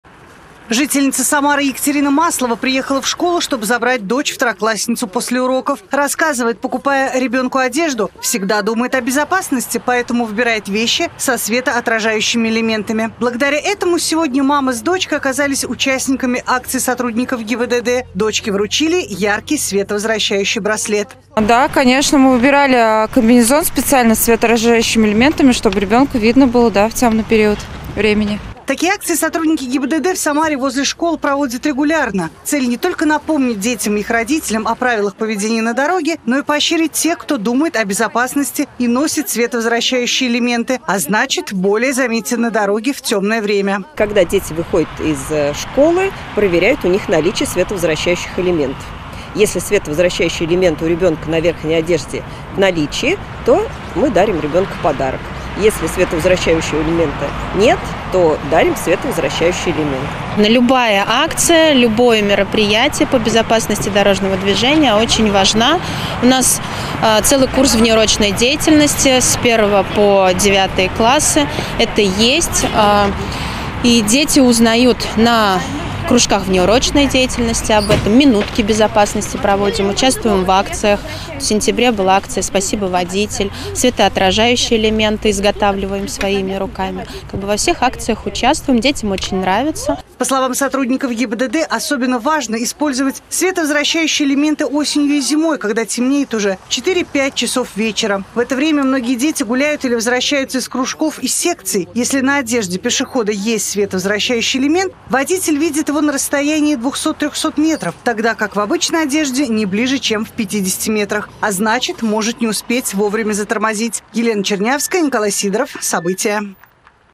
В Самаре прошла акция "Светопатруль"
Уберечь от беды на дороге помогут световозвращающие элементы, которые должны быть правильно закреплены на одежде ребенка. Сегодня сотрудники Госавтоинспекции вручили учащимся 5 школы световозвращающие аксессуары и рекомендовали их использовать в темное время суток, чтобы стать заметными для водителей. Подробнее об акции "Светопатруль" в сюжете Елены Чернявской.